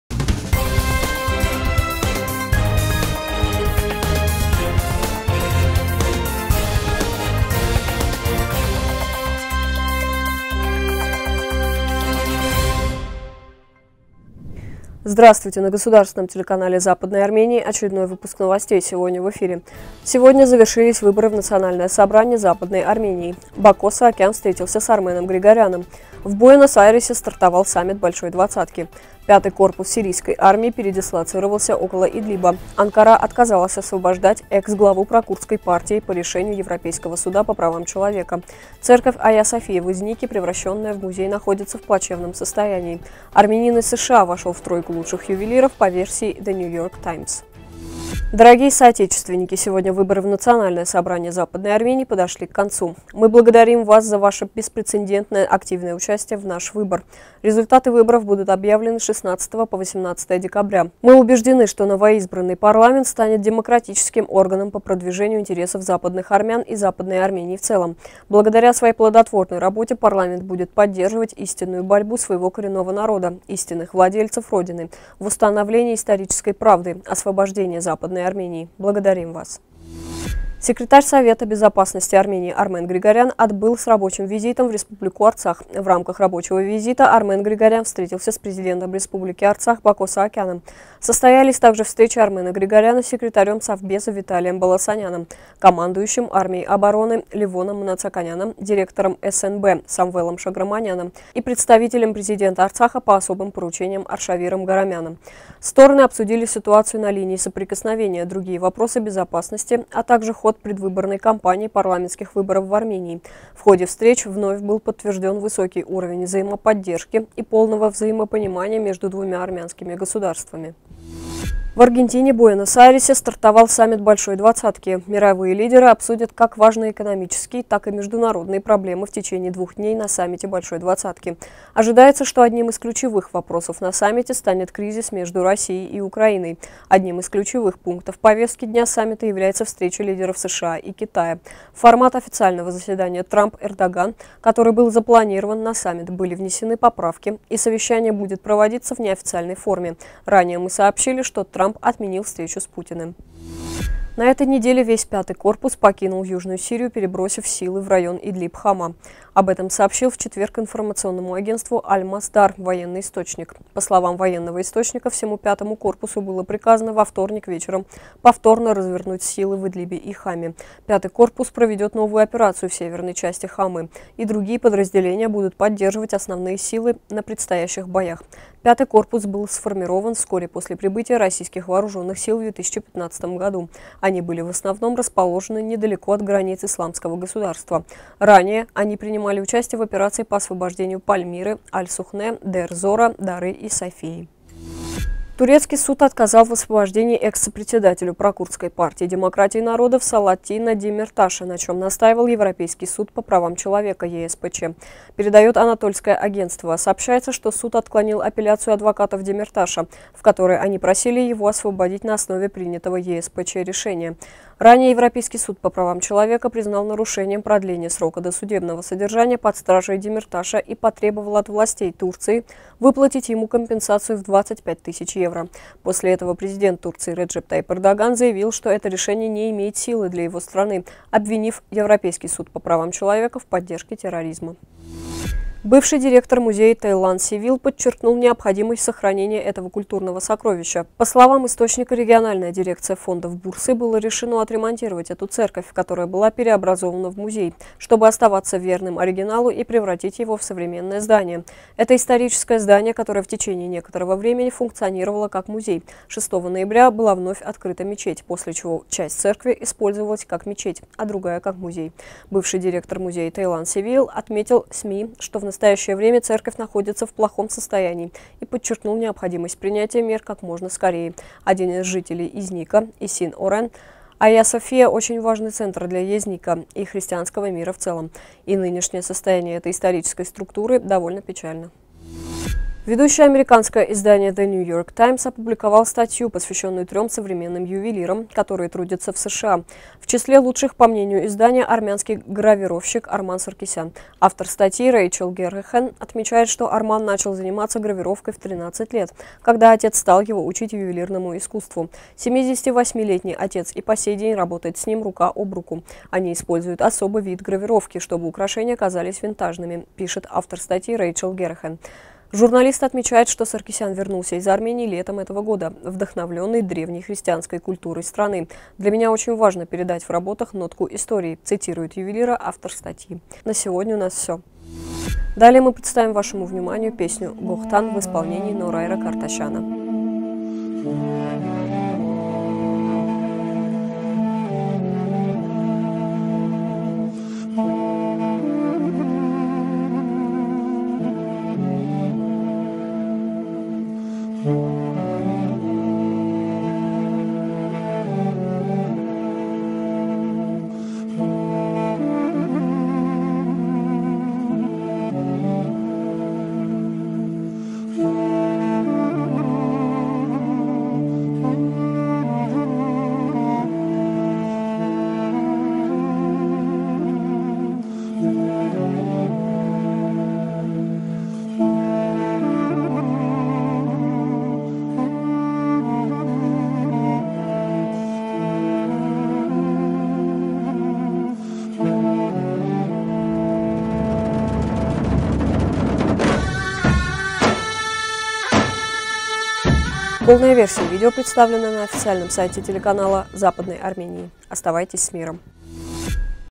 Новости 2018-12-01
1.Сегодня завершились выборы в Национальное Собрание Западной Армении
2.Бако Саакян встретился с Арменом Григоряном
3.В Буэнос Айресе стартовал Саммит G20
4.5-ый корпус сирийской армии передислоцировался около Идлиба
5.Анкара отказалась освободить экс-главу прокурдской партии по решению ЕСПЧ
6․Церковь Айа Софя в Изники превращенная в музей находится в плачевном состоянии
7.Армянин из США вошел в тройку лучших ювелиров по версии The New York Times